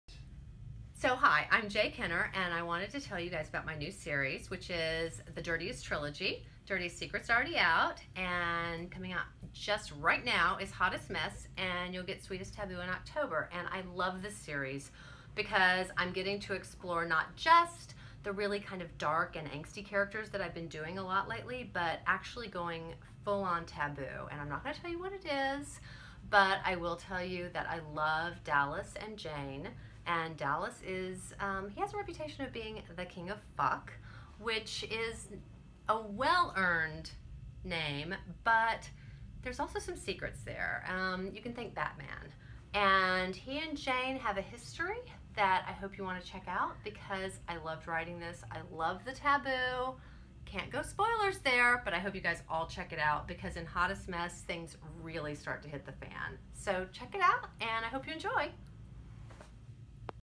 J. Kenner introduces her S.I.N. series
J. Kenner introduces her S.I.N. series (DIRTIEST SECRET, HOTTEST MESS, SWEETEST TABOO).